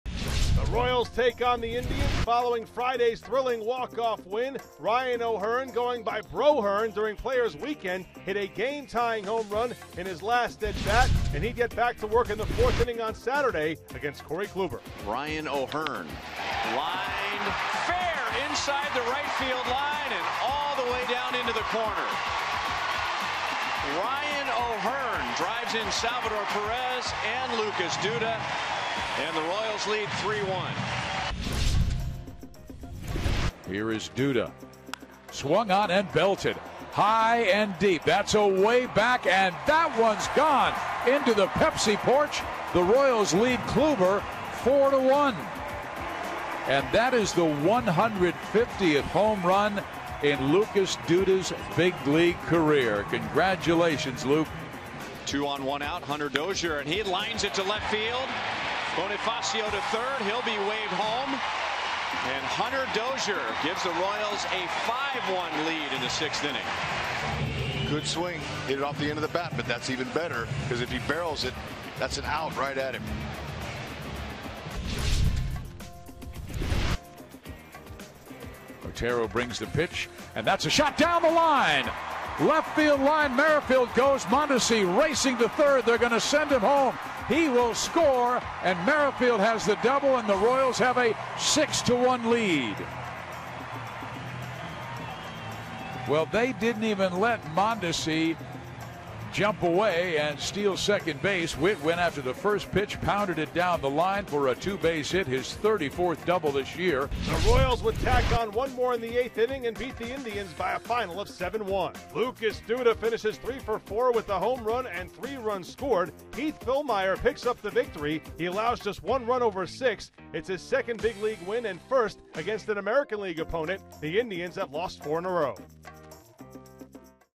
O'Hearn, Fillmyer propel Royals past Tribe: 8/25/18
Daily Recap: Whit Merrifield drove in two runs and Ryan O'Hearn notched three RBIs, as Heath Fillmyer hurled six strong innings in the win